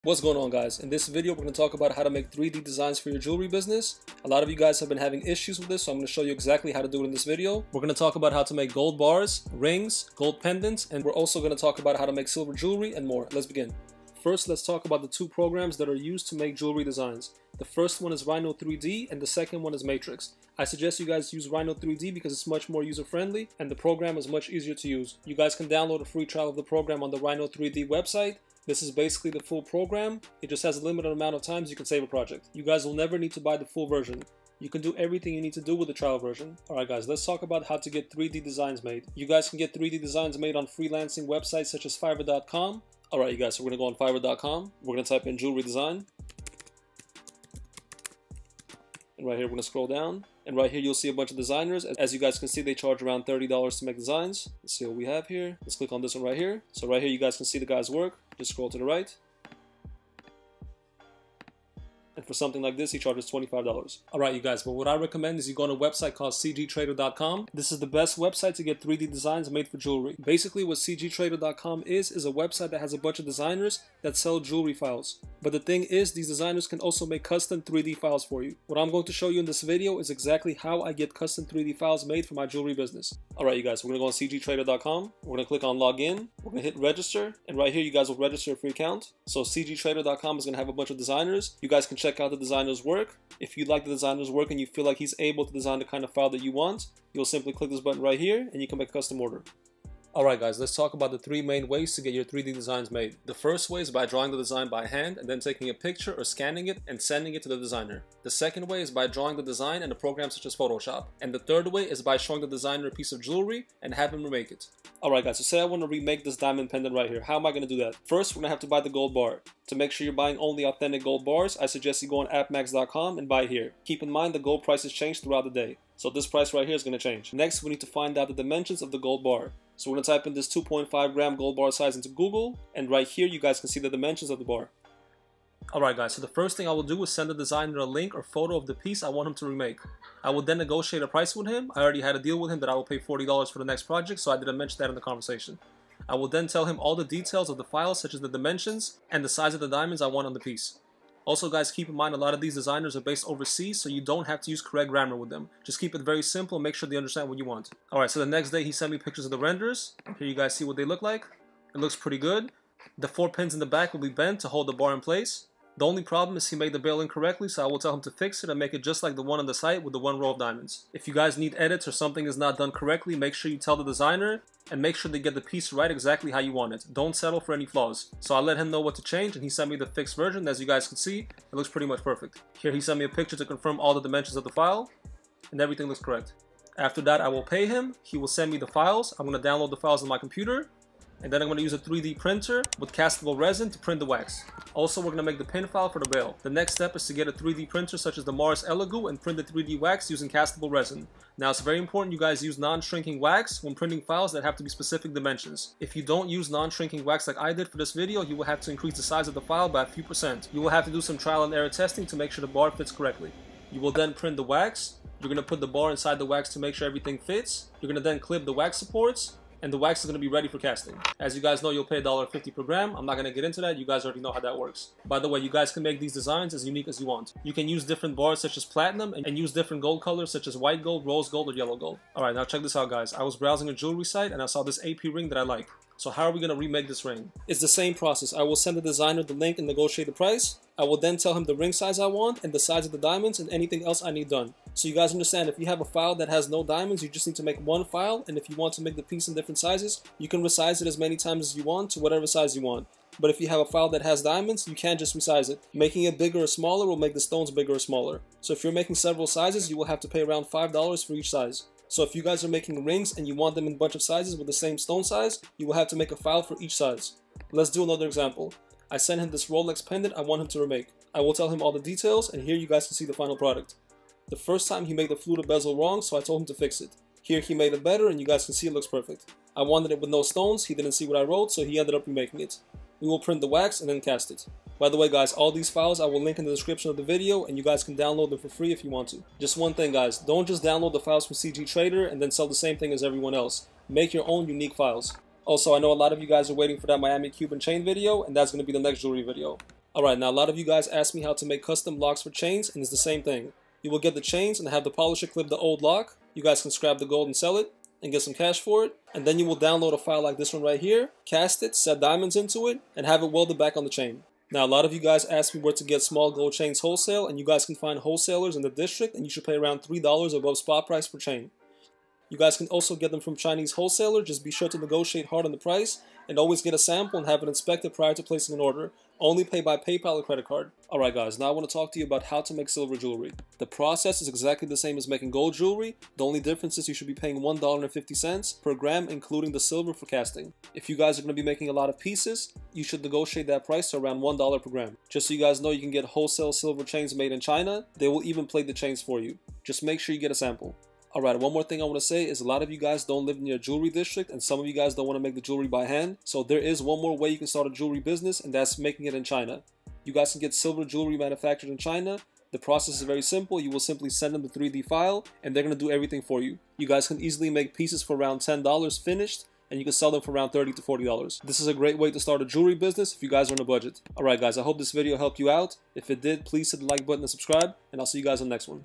How To Get Jewelry Designs Made For Your Jewelry Business (Pamp Suisse Gold Bars, Silver, Rhino 3D)
In this video I will talk about how to make designs for your jewelry business using programs such as Rhino 3D. This is just a quick video guys, I know you want the Miami Cuban ultimate guide thats up next!

Mars Elegoo 2 (new version)